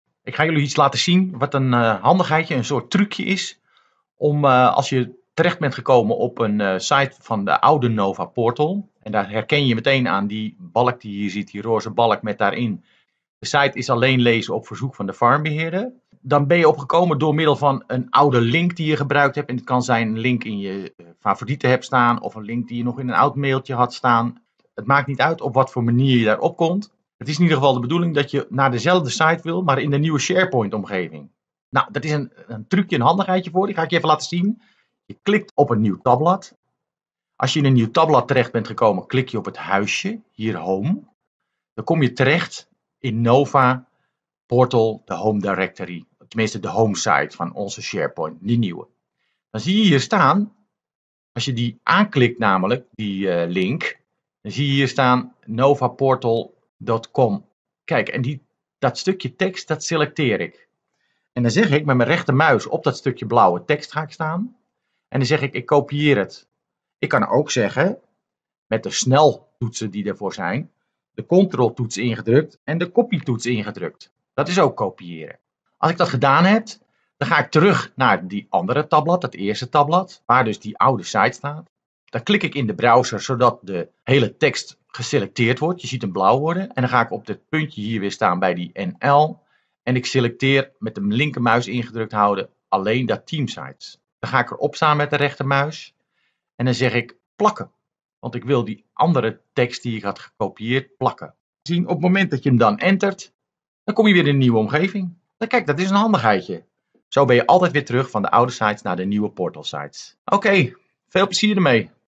Truukje om van oud Portal naar nieuwe SharePoint te gaan
In deze screencast laat ik zien hoe je met een simpel truukje  van de oudePortal site's naar de zelfde  nieuwe SharePoints site gaat